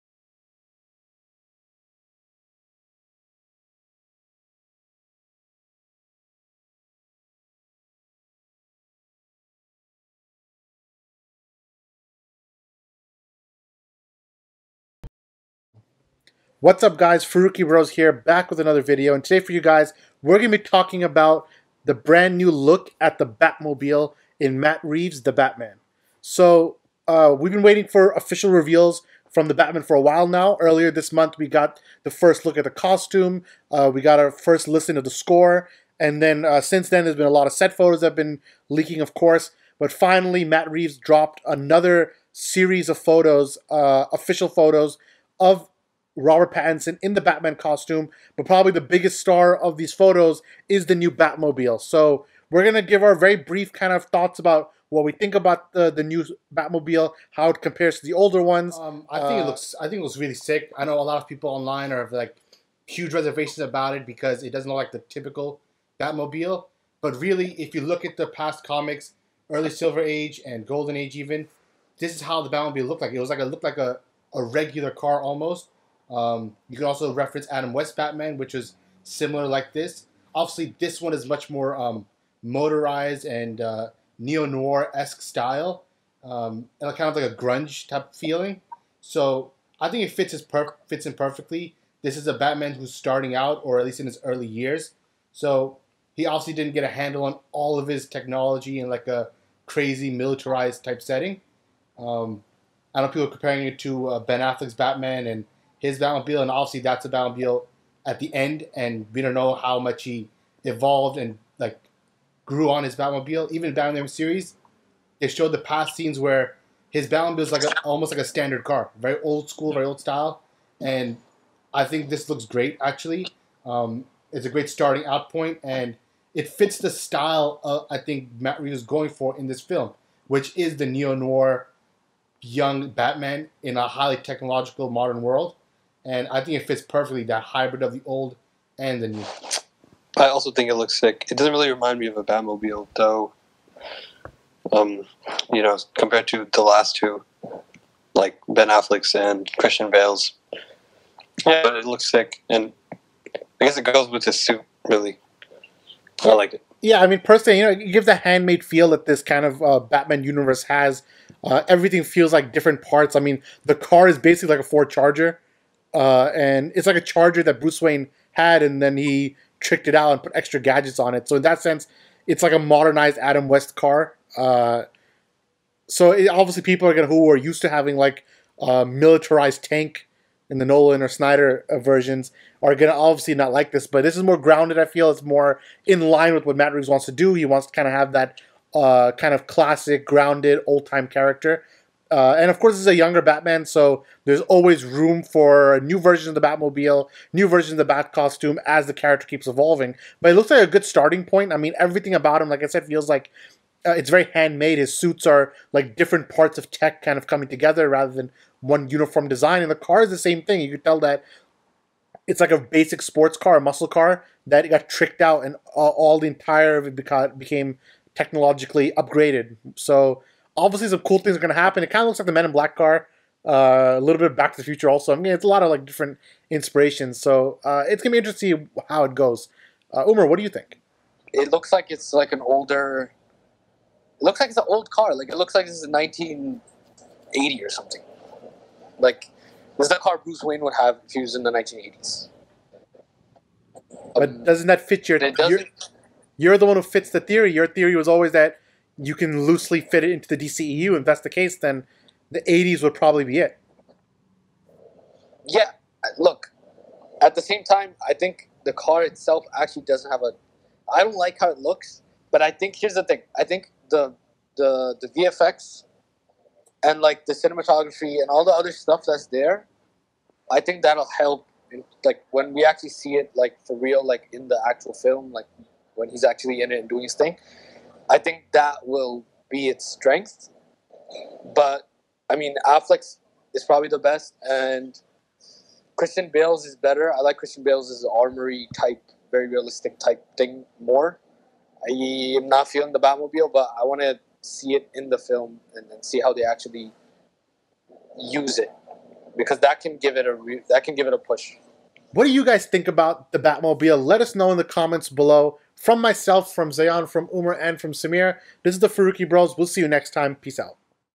FIRST LOOK At The Batmobile for Matt Reeves' THE BATMAN | Which Batmobile Design Works the Best?
ComicBook Debate Reacts to the latest photos of Matt Reeves' The Batman, including the first look at the Batmobile.